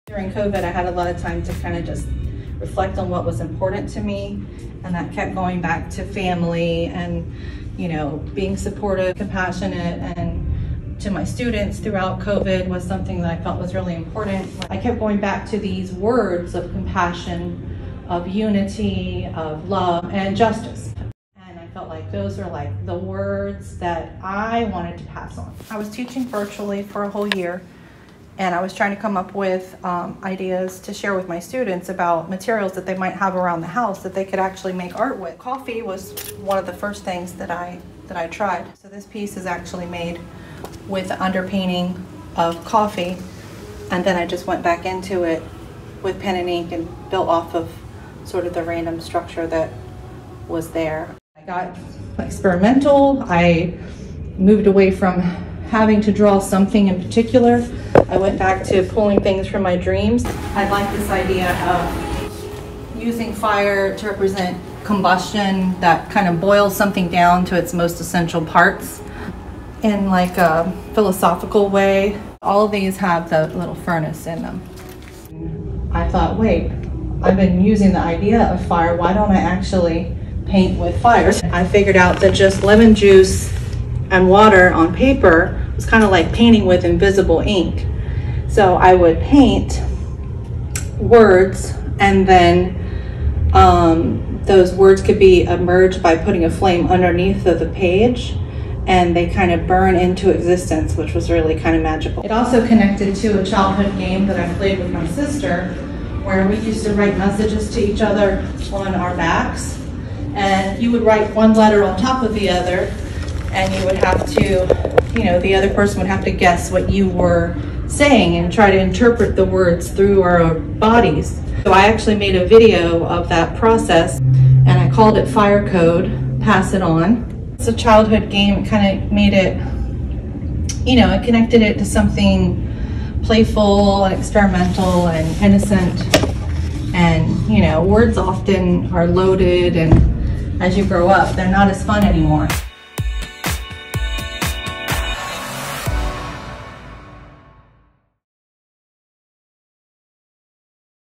2022 Offsite Gallery April Taylor Martin interview - Exhibition on view through 07/01/22
"As an artist, mother and art educator, many parallel concerns have emerged. Through my artwork I am asking the following questions: What is passed on? What transcends our lives and persists in our children and in future generations? What words, moments, and relationships inform our consciousness? What is the role of compassion in education and creativity?

These questions are existential and as such are best explored in artwork with open ended interpretations. Through the process of teaching and creating art I have found that compassion and connection to others allows the transference of thought and meaningful understanding of the world. The characters and symbols in my artworks are playing out these stories. They represent the parts of each of us that give and receive knowledge. They also express some moments that are lost in translation and the confusing result of mixed messages that we all must navigate.

I’m interested in how things beyond words impact our consciousness and are transmitted person to person, moment to moment. Our aesthetic experiences through touch, smell, sound, sight and taste inform our lives in ways that are difficult to comprehend. These artworks explore that process of seeking knowledge that transcends generations, time and individuals, and tries to connect with essential truths about our "Cognition Transmission". Through this body of work I will investigate these questions about what we Pass On, and open up the experience to viewers through discussion and participation."

April is the Senior Supervisor of Visual Arts for Portsmouth Public Schools. She received a Master of Fine Arts Degree from Norfolk State University and Old Dominion University and a Bachelor’s Degree of Fine Arts from Old Dominion University.